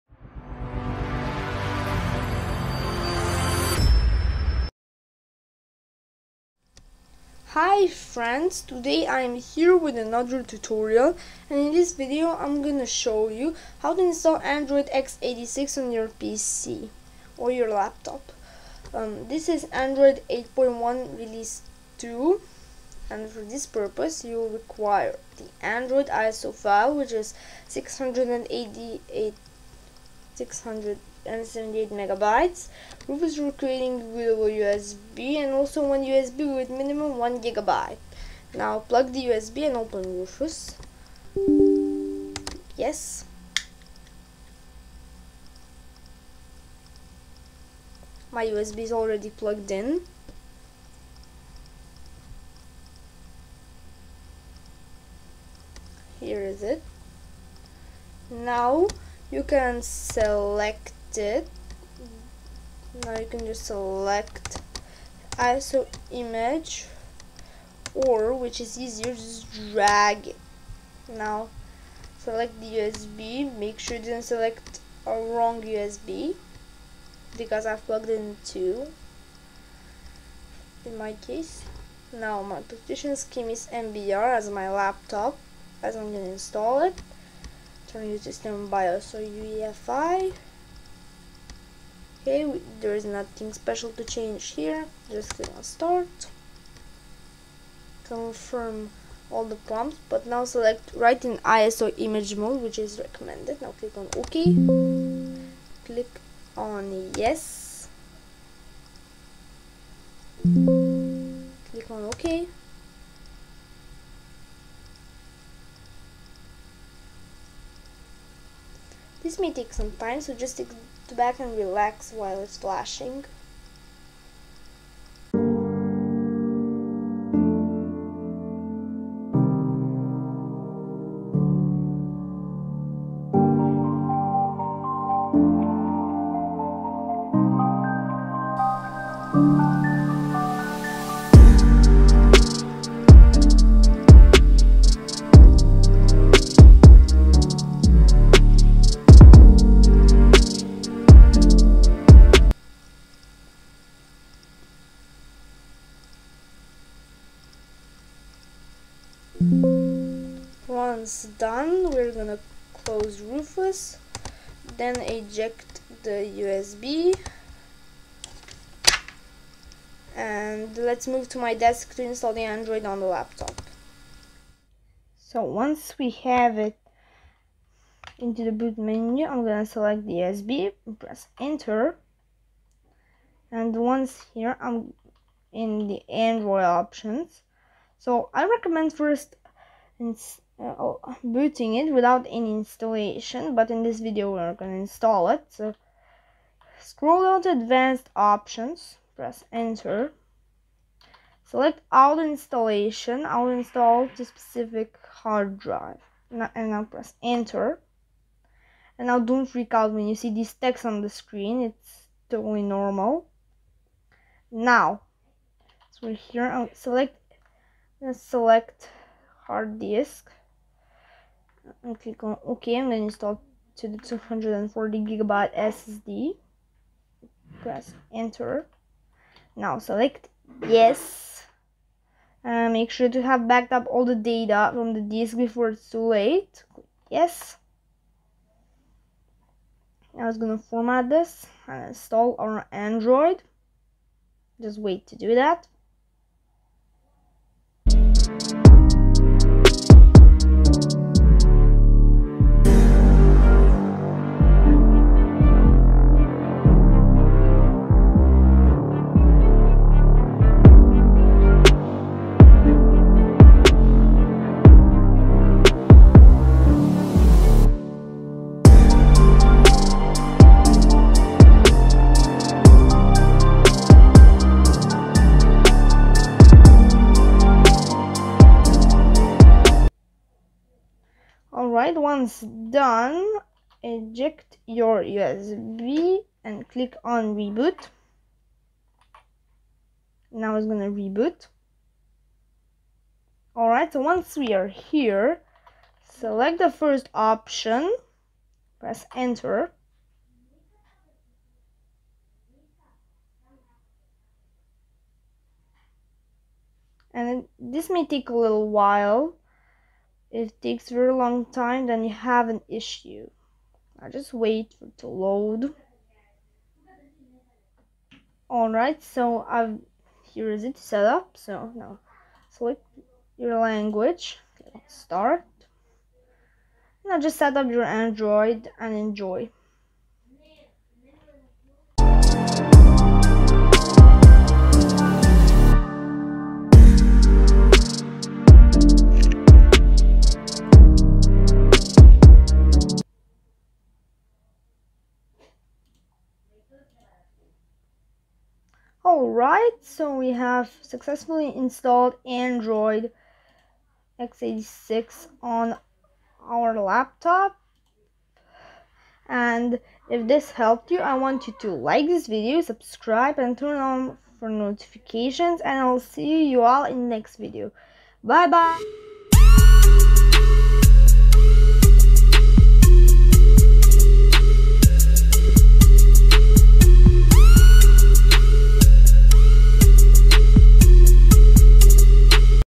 How To Install Android x86 (version 8.1) on your PC or Laptop
Hi, friends! Today, I'm gonna show you how to install Android x86 on your PC/Laptop

Timestamps

00:00 Intro
00:19 What you need for this operation
00:37 Making bootable USB
02:54 Installing Android 8.1
04:50 First boot
05:18 Setting up Android 8.1
05:49 First look
06:00 Outro

Note: When I said 240 GB SSD I made a mistake, the SSD is 256 GB